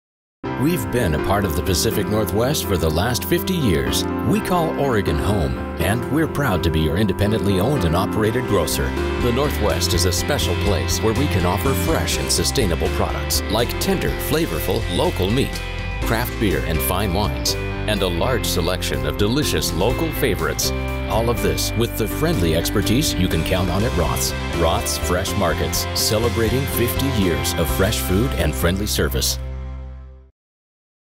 Roth's Fresh Market's 50th Anniversary Celebration
At Roth's, we're proud to have been part of the Pacific Northwest for the last 50 years...

Oregon is a special place and we feel lucky to be your independently owned and operated grocer. From the rich waters of the Pacific coast to the fertile lands right here in the Willamette Valley and all over the Northwest we live in a place that lets us live fresh, and sustainably... That's why at Roth's we're proud to offer so many local products...